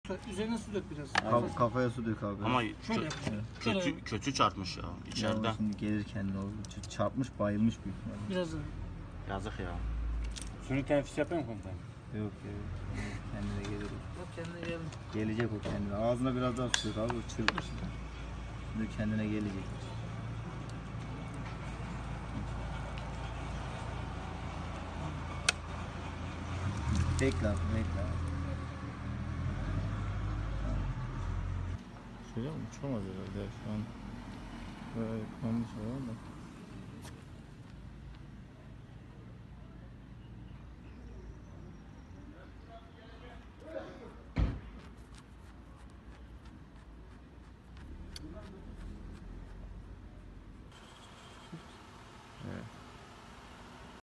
Bingöl'de Mehmetçik’ten bayılan serçeye can suyu
Bingöl’de göreve giden Mehmetçik, baygın halde bulduğu serçeyi verdiği can suyu ile sağlığına kavuşturdu. Haber detay